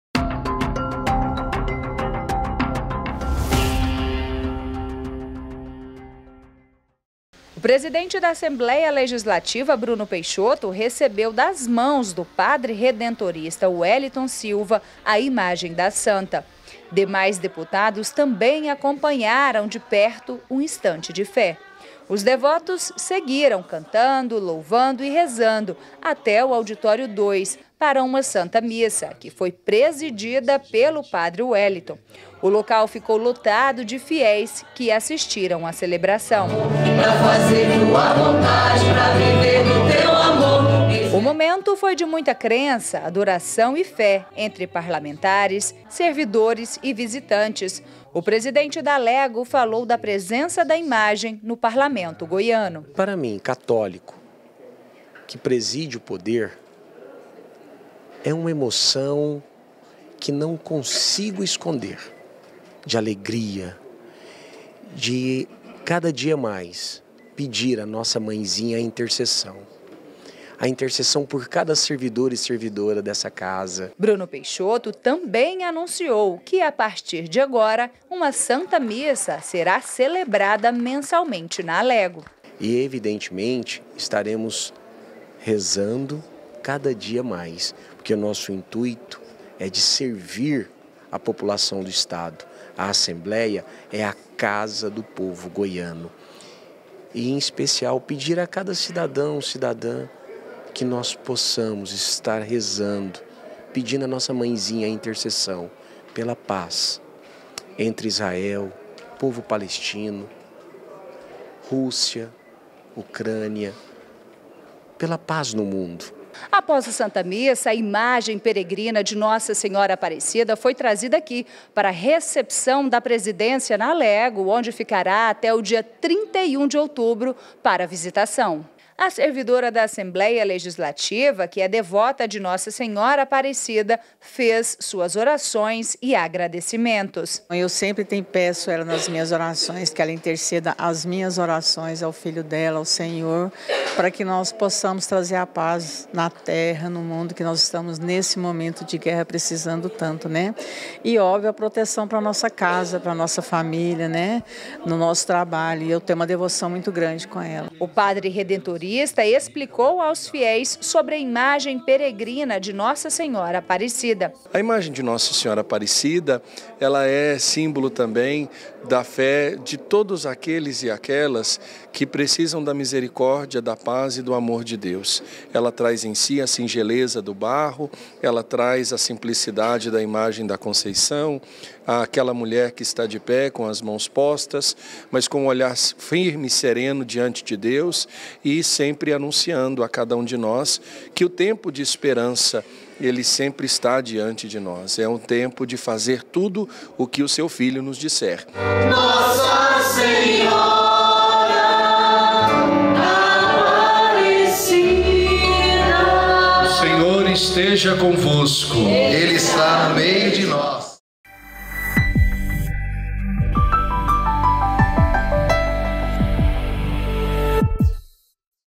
Alego News: Chegada da imagem peregrina de Nossa Senhora Aparecida (18/10/2023)
Com balões azuis e brancos, dezenas de servidores da ALEGO aguardaram a chegada da imagem peregrina de Nossa Senhora Aparecida. O momento foi de muita fé, amor e agradecimento à padroeira do Brasil.

O presidente da Assembleia Legislativa, Bruno Peixoto, recebeu das mãos do padre redentorista Weliton Silva a imagem da santa. Demais deputados também acompanharam de perto o instante de fé. Os devotos seguiram cantando, louvando e rezando até o auditório 2, para uma Santa Missa, que foi presidida pelo padre Weliton. O local ficou lotado de fiéis que assistiram à celebração.

O momento foi de muita crença, adoração e fé entre parlamentares, servidores e visitantes. O presidente da ALEGO falou com a TV Alego sobre a presença da imagem no parlamento goiano.
Bruno Peixoto também anunciou que, a partir de agora, uma Santa Missa será celebrada mensalmente na ALEGO

A servidora da Assembleia Legislativa, Mercione Araújo que é devota de Nossa Senhora Aparecida, fez suas orações e agradecimentos

O padre redentorista, Weliton Silva,  explicou na reportagem e aos fiéis sobre a imagem peregrina de Nossa Senhora Aparecida

Veja os destaques com a repórter Thiara Castilho.